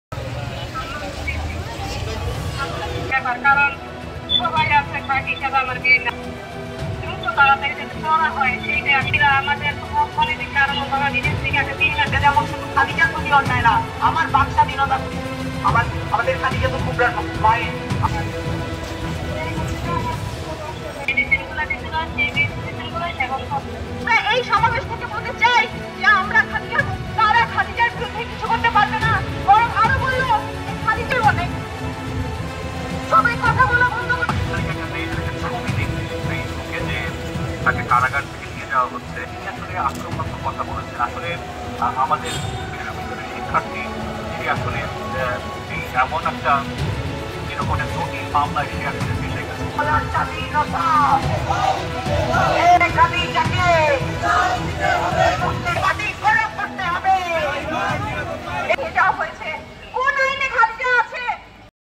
শিক্ষার্থীরমুক্তির দাবিতে বিক্ষোভ
ডিজিটাল নিরাপত্তা আইনের মামলায় কারাবন্দী জগন্নাথ বিশ্ববিদ্যালয়ের শিক্ষার্থী খাদিজাতুল কুবরার মুক্তির দাবিতে ক্যাম্পাসে মানববন্ধন ও বিক্ষোভ কর্মসূচি পালন করেন জগন্নাথ বিশ্ববিদ্যালয়ের শিক্ষার্থীরা।

জগন্নাথ বিশ্ববিদ্যালয়ের শিক্ষার্থীবৃন্দের’ ব্যানারে কর্মসূচিতে বিভিন্ন বিভাগের শিক্ষার্থীরা অংশ নেন। মানববন্ধনে শিক্ষার্থীরা বলেন, ডিজিটাল নিরাপত্তা আইনের কারণে যদি গণমাধ্যম ও সাধারণ মানুষ আতঙ্কে থাকে, তাহলে এই আইন কোনোভাবে ভালো হতে পারে না। তাঁরা ডিজিটাল এ আইনের বাতিলের দাবি জানান।